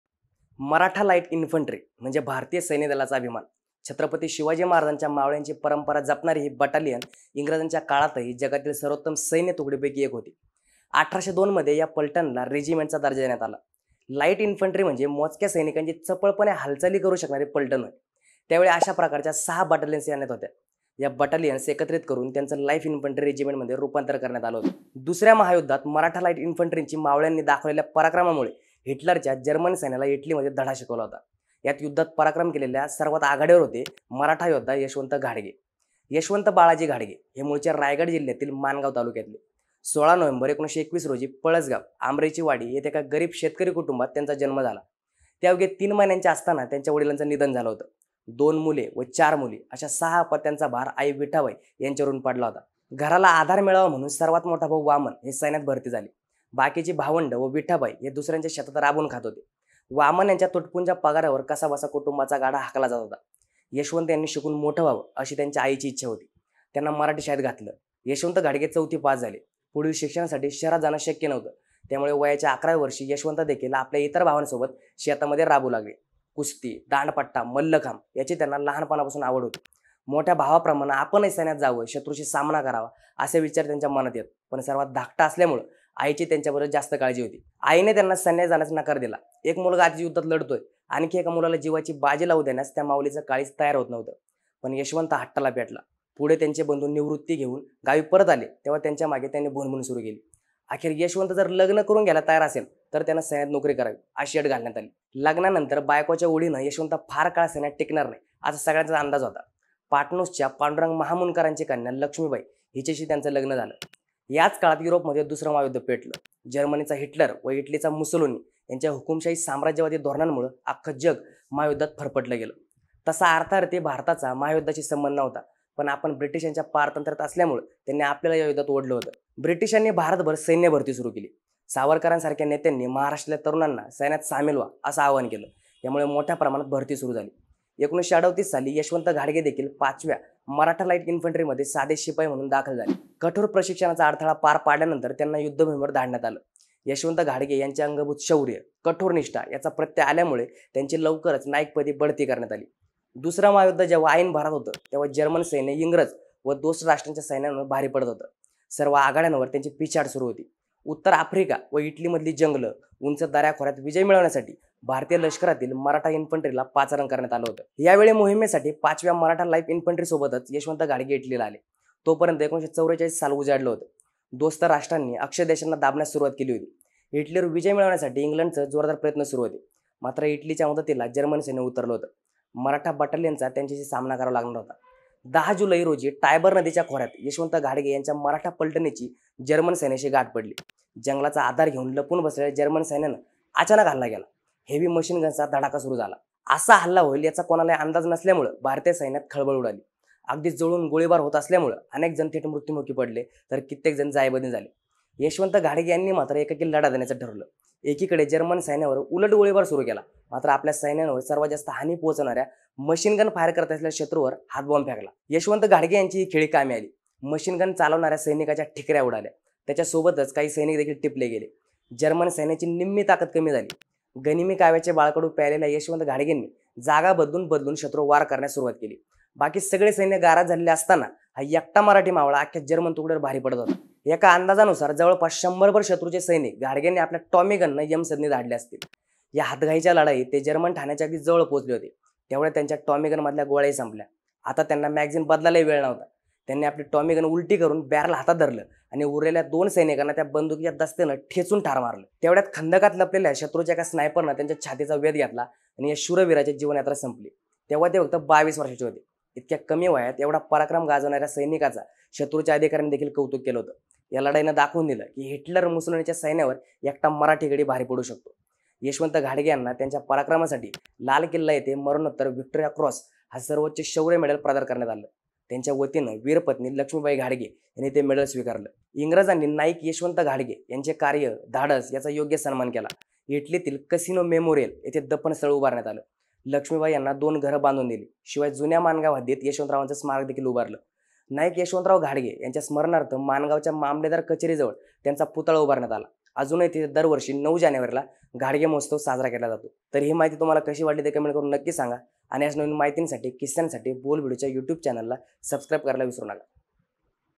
यशवंतराव घाडगेंच्या इटलीतल्या पराक्रमासाठी रायगड जिल्ह्यात त्यांचा पुतळा उभारण्यात आला | Bol Bhidu
लाईट इंन्फंट्री म्हणजे मोजक्या सैनिकांची चपळपणे हालचाली करू शकणारी पलटण होय. त्यावेळी अशा प्रकारच्या सहा बटालियन्स सैन्यात होत्या. या बटालियन्स एकत्रित करून त्याचे लाईट इन्फंट्री रेजिमेंटमधे रुपांतर करण्यात आले.

दुसऱ्या महायुद्धात मराठा लाईट इन्फन्ट्रीच्या मावळ्यांनी दाखवलेल्या पराक्रमामुळे हिटलरच्या जर्मन सैन्याला इटलीमध्ये धडा शिकवला होता.
यात युद्धात पराक्रम केलेल्या मध्ये सर्वात आघाडीवर नाव येते मराठा योद्धा यशवंत घाडगे यांचं.

The Light Infantry is a regiment that can move a small number of soldiers. At that time there were six such battalions in the army. These battalions were amalgamated into the Light Infantry Regiment.

The heroism of the Maratha Light Infantry in World War II had taught Hitler's German army a lesson in Italy.
Maratha warrior Yashwant Ghadge is one of the foremost war heroes.